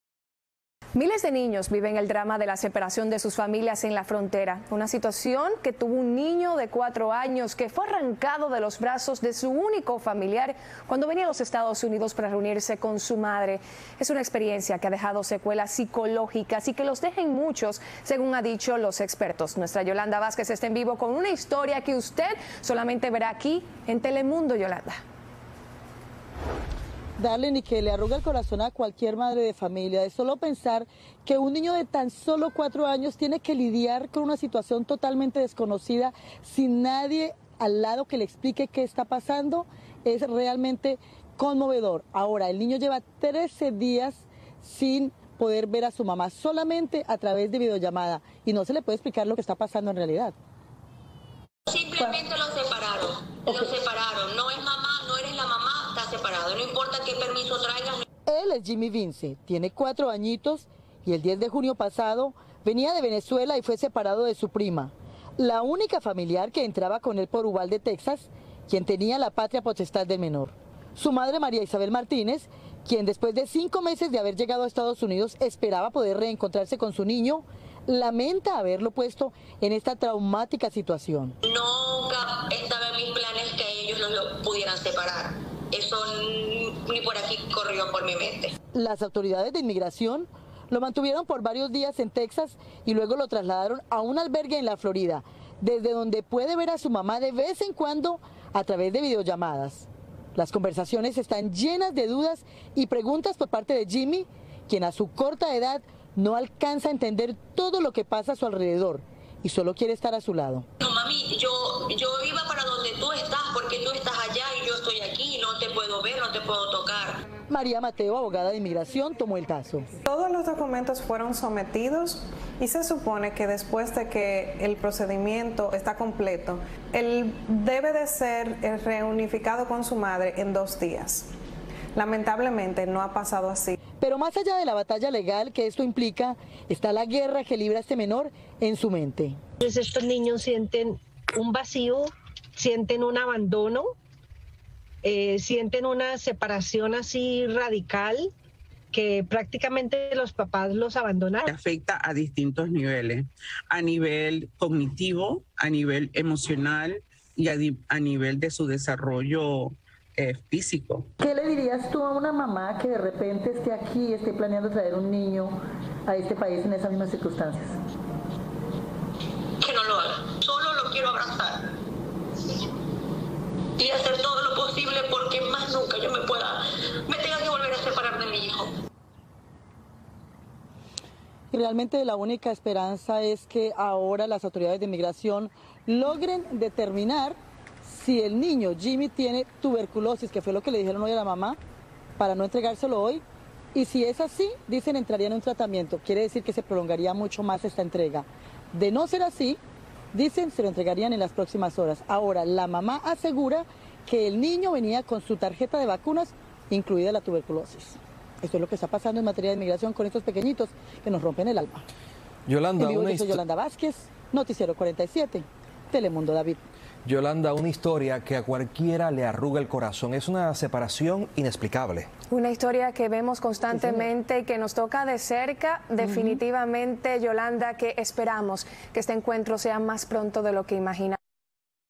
Un pequeño de 4 años fue separado de su familia en la frontera. Vea lo que ocurrio.
Our 4 years old client was separated from his family at the border.
Nuestro cliente de 4 años fue separado de su familia en la frontera.